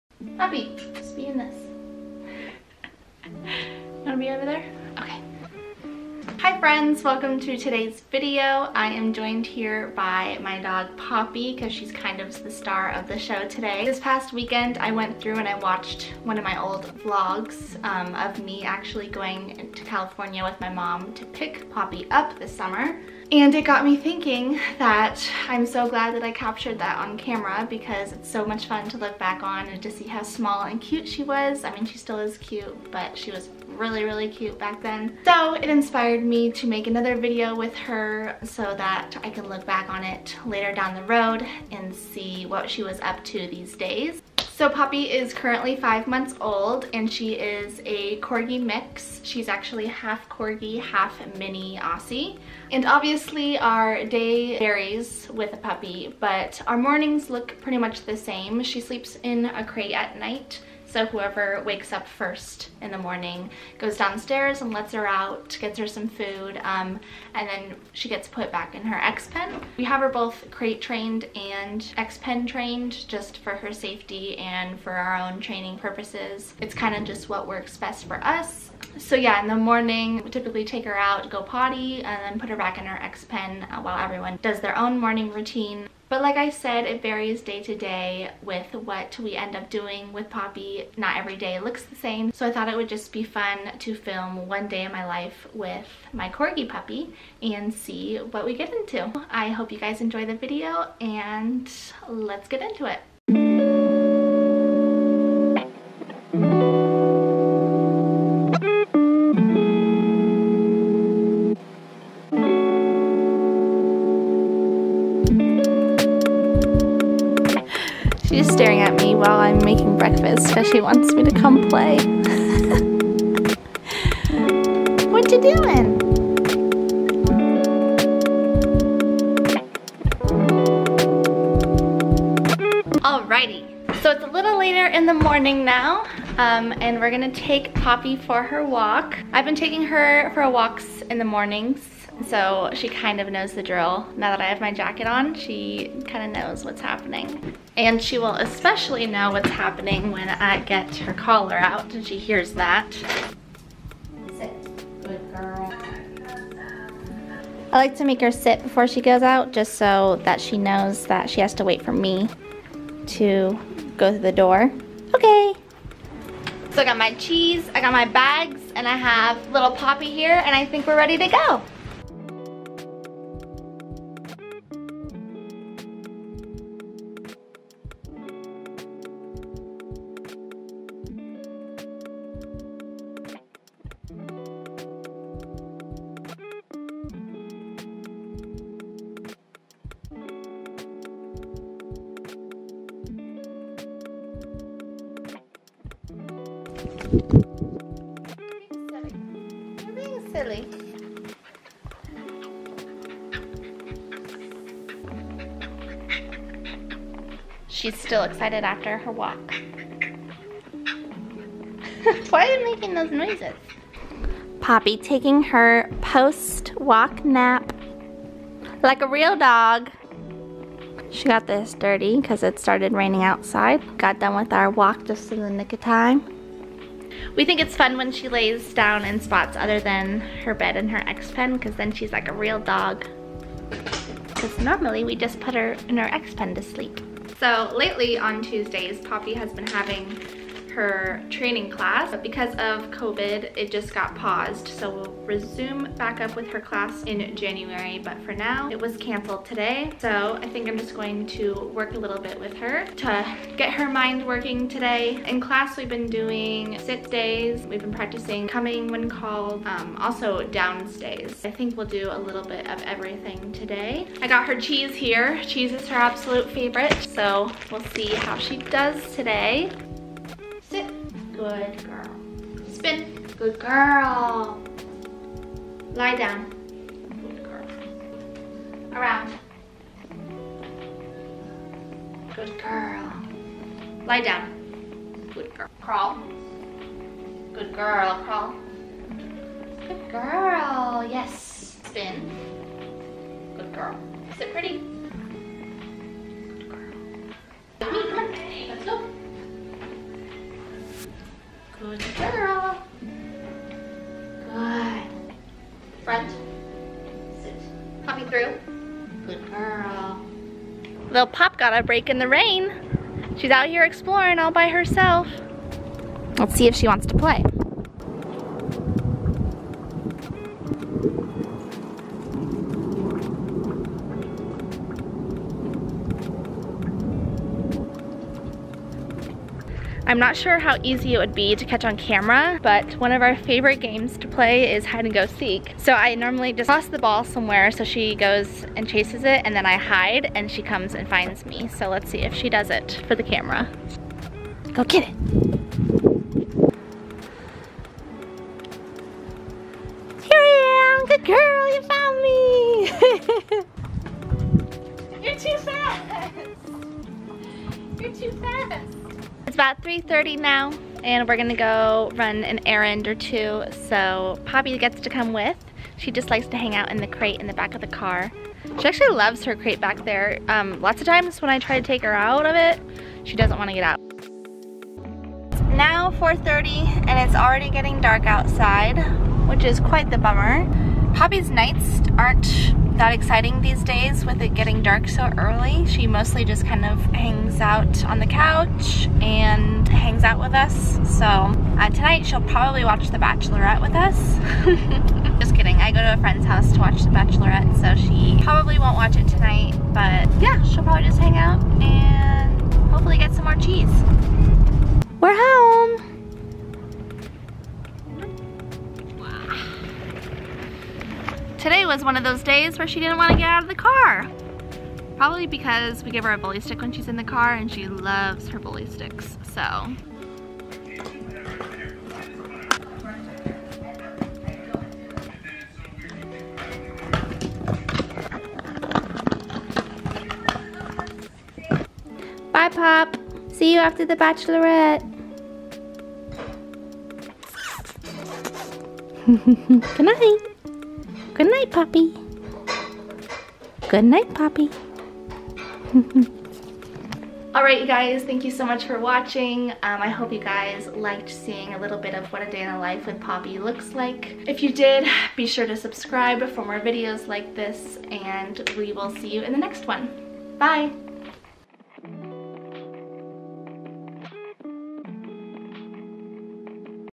A Day in the Life with a Corgi (mix) Puppy!
Hi friends! My puppy Poppy is seriously growing by the minute, so I thought it would be fun to capture what a day in the life is like with her at 5 months old. It was a pretty typical day for us; we go for a walk, do some training, play hide & seek in the backyard, and more! Poppy is a light in our lives, so i hope she brings you some joy today, too!